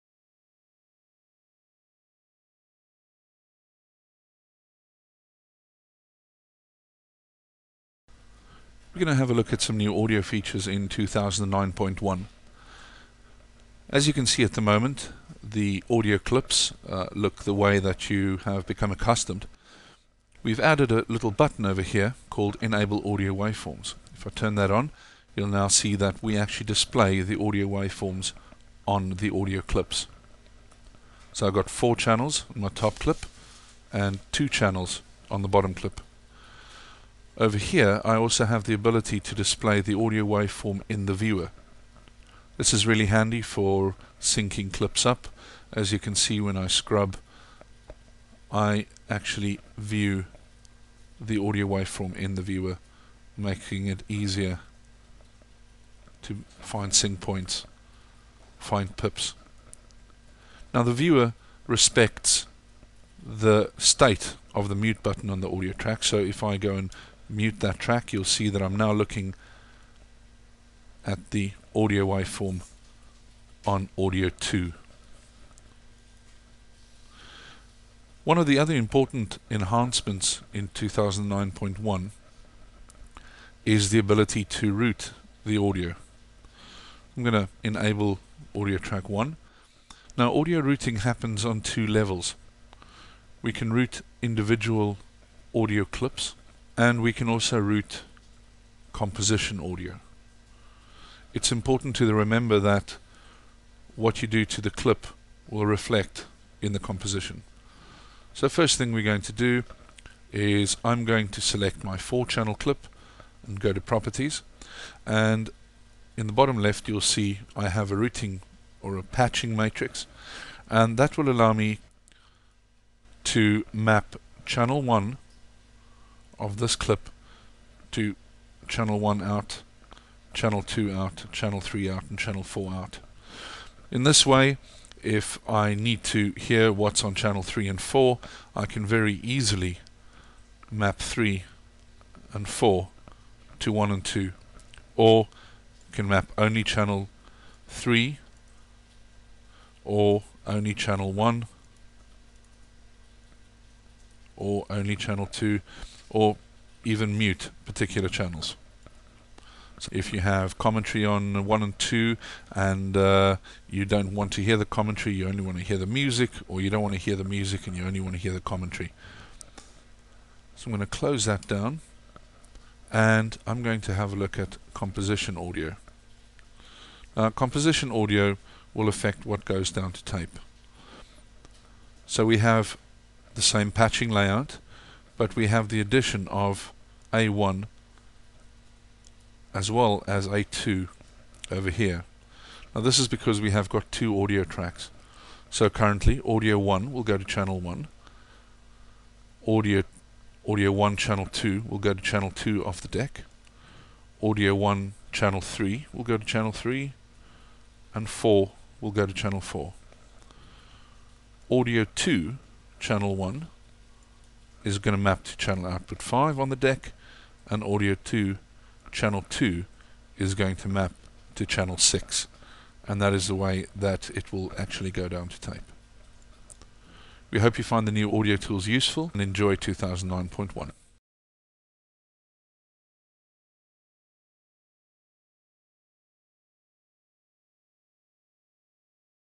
Nucoda 2009.1 Sofware - Audio Enhancements
Overview the latest audio enhancements as part of the Nucoda 2009.1 software release.